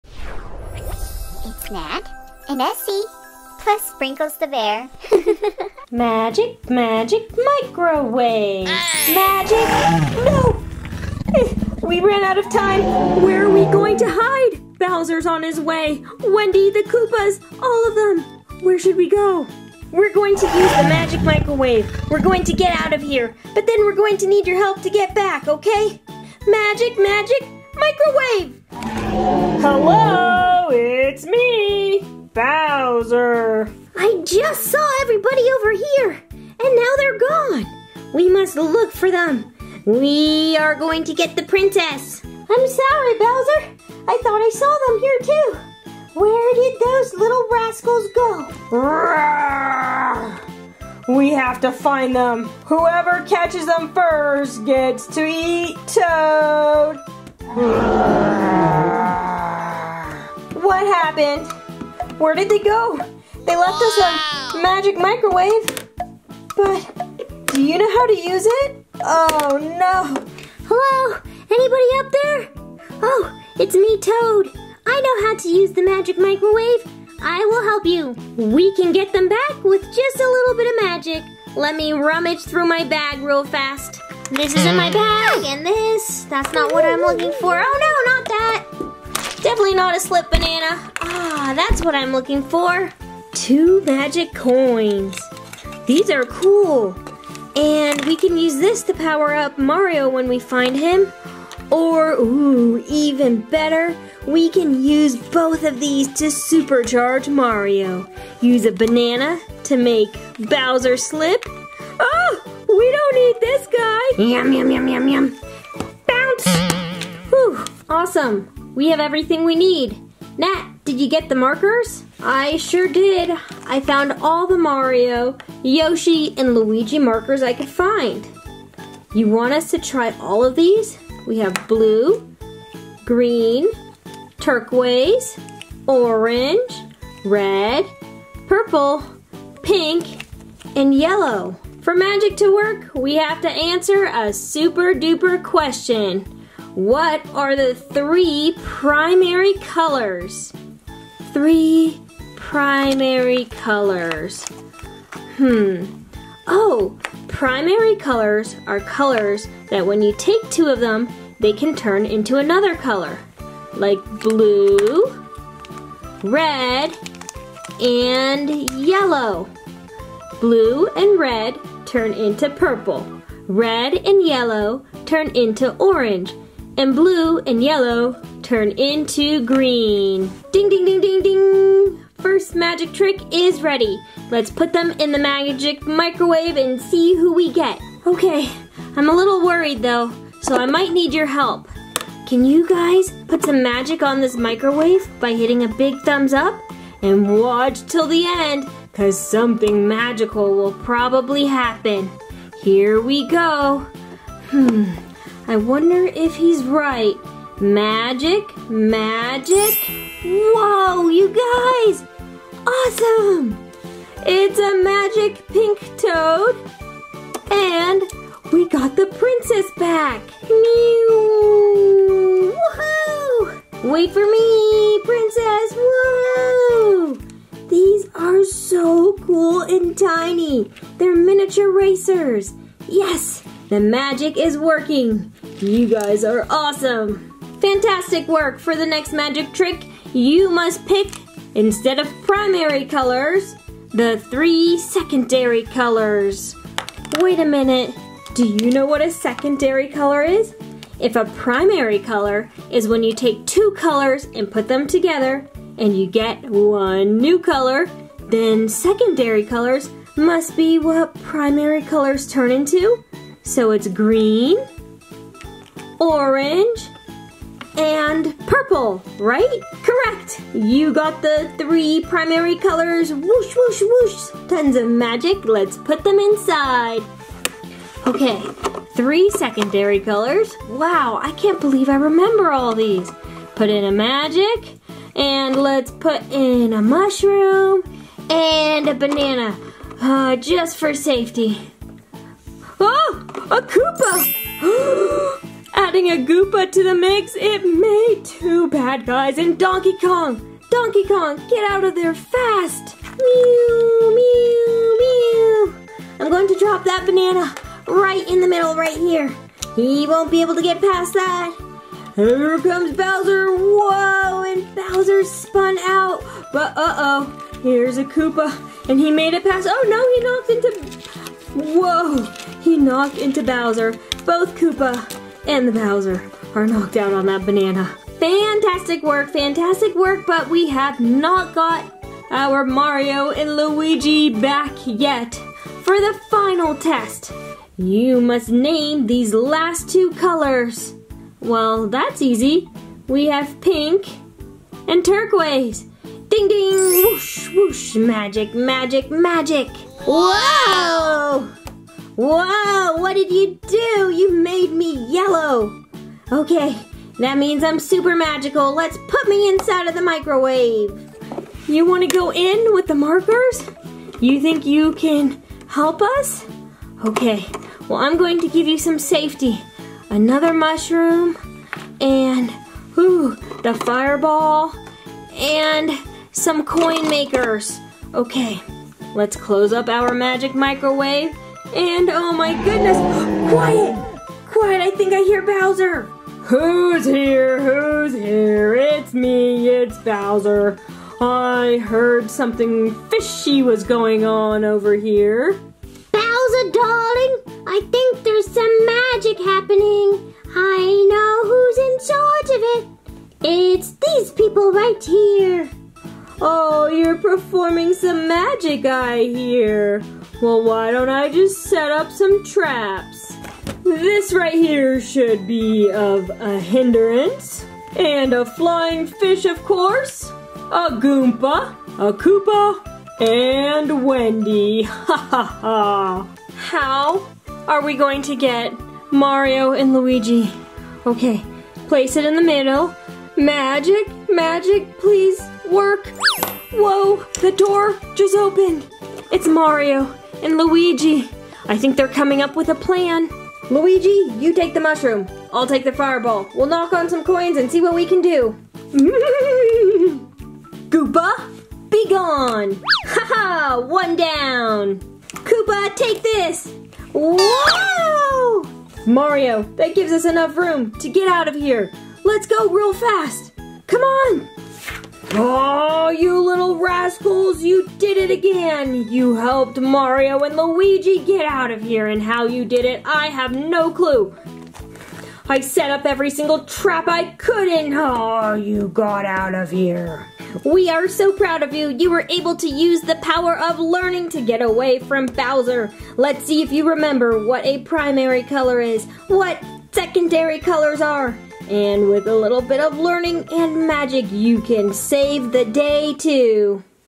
The Super Mario Bros Movie Magic Microwave with Bowser and Toad
The Super Mario Bros Movie Magic Microwave with Bowser & Luigi

FTC- I am not being paid by any of the mentioned companies  to make this video.